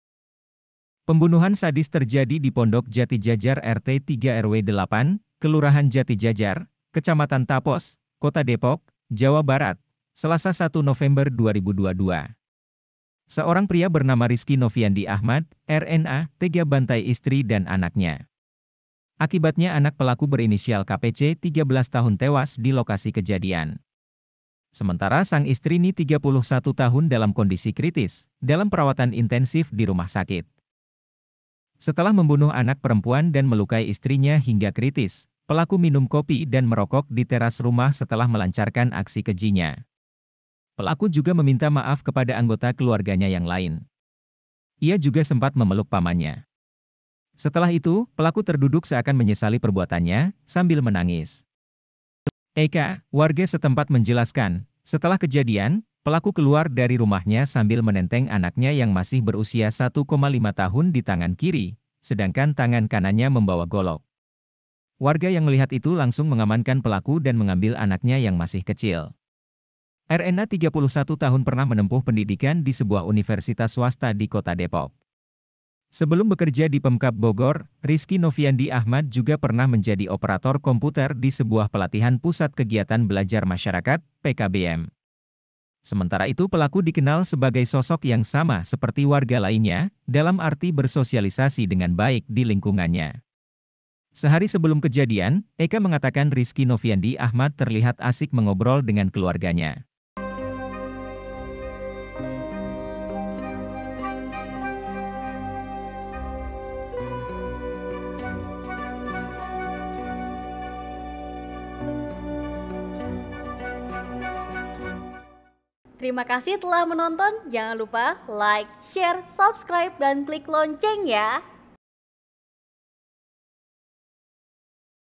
🔴 Sosok RNA, Pria di Depok yang Habisi Nyawa Anaknya Sendiri
Pembunuhan sadis terjadi di Pondok Jatijajar RT 3 RW 8, Kelurahan Jatijajar, Kecamatan Tapos, Kota Depok, Jawa Barat, Selasa 1 November 2022. Seorang pria bernama Rizky Noviyandi Achmad (RNA) tega bantai istri dan anaknya.

Akibatnya anak pelaku berinsial KPC (13) tewas di lokasi kejadian. Sementara sang istri NI (31) dalam kondisi kritis, dalam perawatan intensif di rumah sakit.

Saat ini, pelaku telah diamankan di Polres Metro Depok untuk dimintai keterangan atas aksi kejinya. Belum diketahui secara pasti motif pelaku tega menganiaya istri dan anaknya.

Setelah membunuh anak perempuan dan melukai istrinya hingga kritis, pelaku minum kopi dan merokok di teras rumah setelah melancarkan aksi kejinya.

Pelaku juga meminta maaf kepada anggota keluarganya yang lain. Ia juga sempat memeluk pamannya. Setelah itu, pelaku terduduk seakan menyesali perbuatannya, sambil menangis.

Eka, warga setempat menjelaskan, setelah kejadian, pelaku keluar dari rumahnya sambil menenteng anaknya yang masih berusia 1,5 tahun di tangan kiri, sedangkan tangan kanannya membawa golok.

Warga yang melihat itu langsung mengamankan pelaku dan mengambil anaknya yang masih kecil. RNA (31) pernah menempuh pendidikan di sebuah universitas swasta di Kota Depok.

Menurut salah seorang tetangga, Eka Heri, pelaku pembunuhan sadis tersebut sehari-hari bekerja sebagai tenaga honorer di Kantor Pemerintahan Daerah Kabupaten Bogor.

Sebelum bekerja di Pemkab Bogor, Rizky Noviyandi Achmad juga pernah menjadi operator komputer di sebuah Pelatihan Pusat Kegiatan Belajar Masyarakat (PKBM).

Sementara itu pelaku dikenal sebagai sosok yang sama seperti warga lainnya, dalam arti bersosialisasi dengan baik di lingkungannya. Sehari sebelum kejadian, Eka mengatakan Rizky Noviyandi Achmad terlihat asik mengobrol dengan keluarganya.